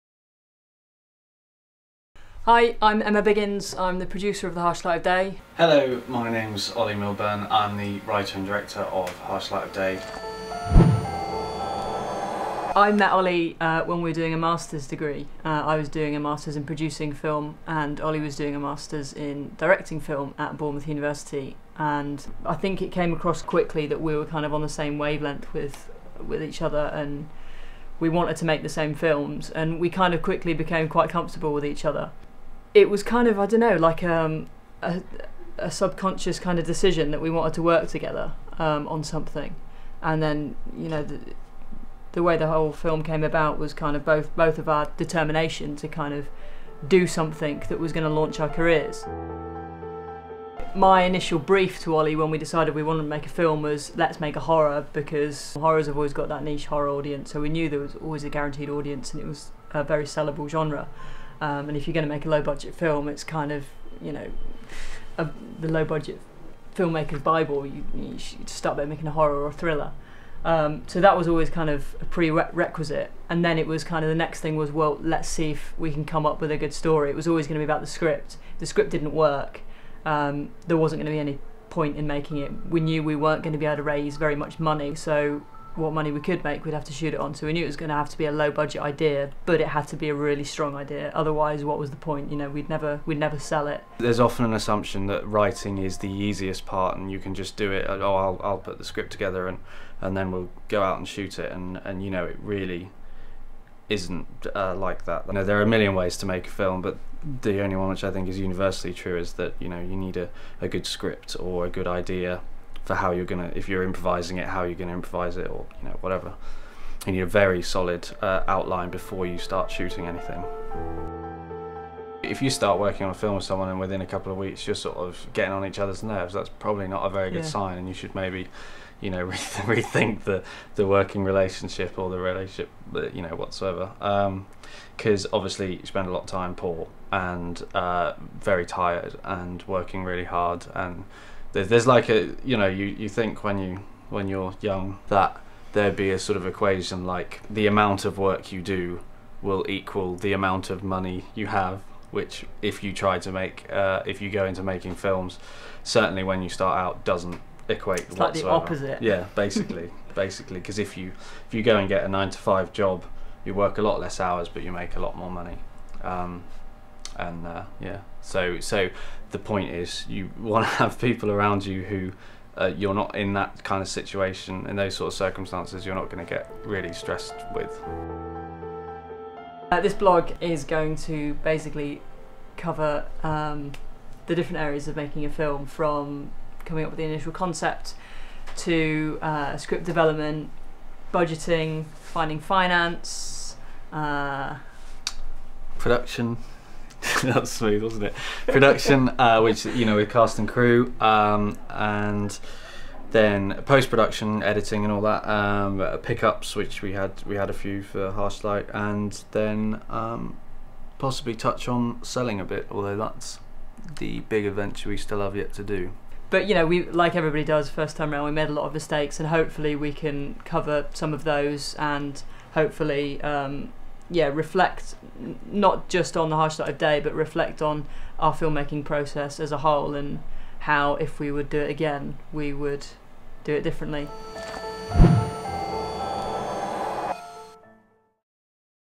The Harsh Light of Day Blog No. 1.mov
The first in a series of blog posts, the producer and writer/director team behind The Harsh Light of Day explain why they made the film and what you can expect from this blog over the coming weeks.

Each post will look at a different stage in the film's production from both points of view. Filled with honest and insightful information, the blog will be of interest to any film enthusiast or budding filmmaker.

So press play to see what it is all about.